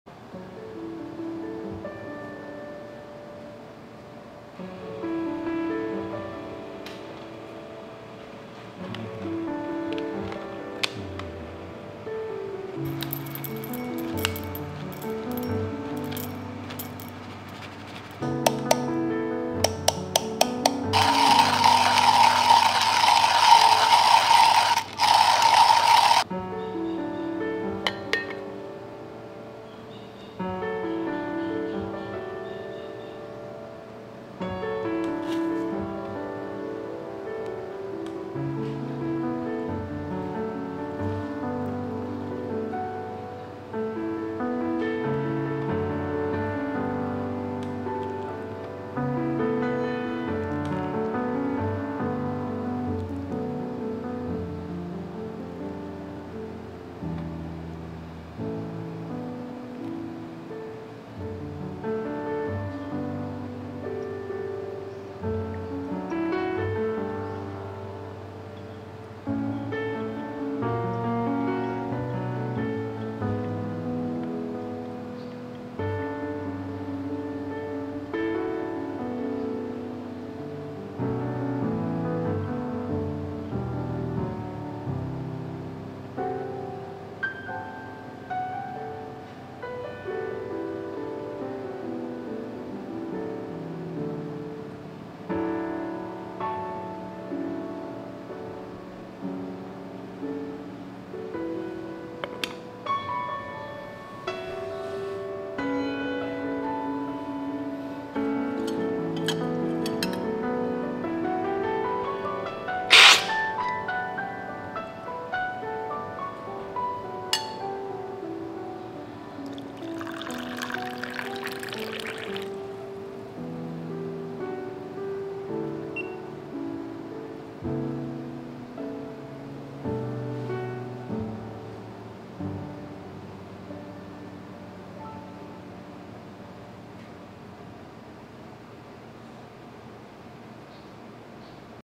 Mornings with Origami Flat Bottom Filter
Grinder: Comandante C40
Brewer: Origami Air S
Filter: Kalita Wave 155
Server: Hario 01
Scale: DiFluid Microbalance
Kettle: Brewista Artisan Kettle

Coffee: Blk & Bold Limu Geisha